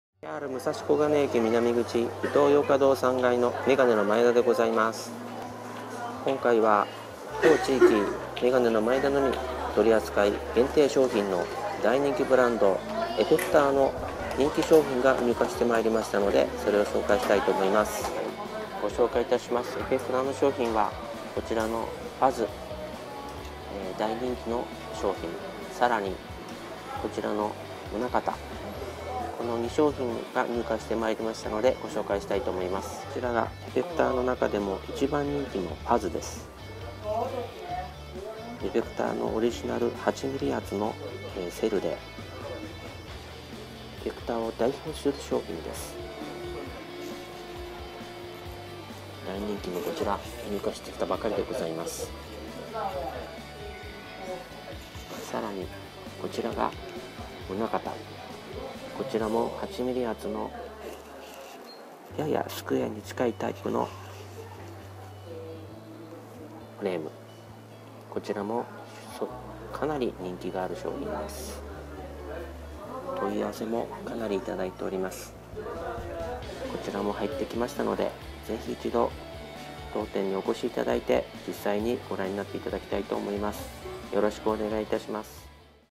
EFFECTOR の人気商品 fuzz と MUNAKATA待望の入荷 (武蔵小金井 メガネ)
EFFECTORを代表する人気の商品。fuzzは俳優の妻夫木 聡さんも愛用。福井県鯖江の職人さんが一つ一つ手作り。今回待望の入荷。その質感をぜひ店頭でお確かめ下さい。